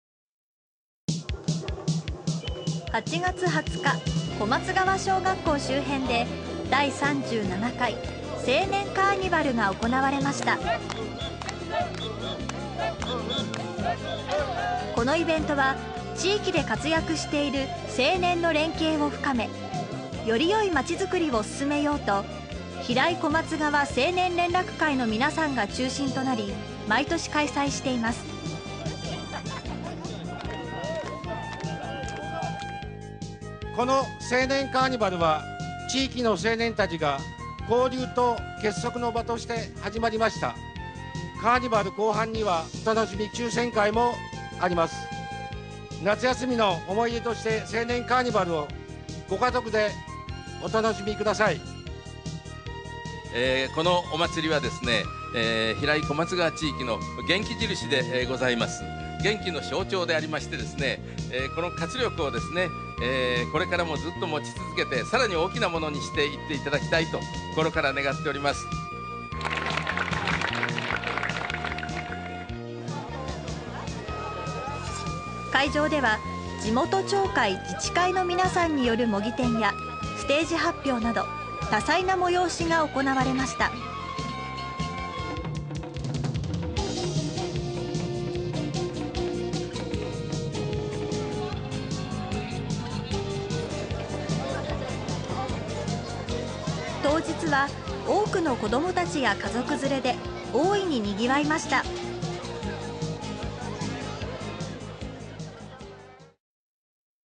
第37回 青年カーニバル
このイベントは、平井小松川青年連絡会の皆さんが中心となり毎年開催しているおまつりです。名物の万灯みこしパレードや楽しげなおまつりの様子をご覧ください。　(平成29年9月1日 公開)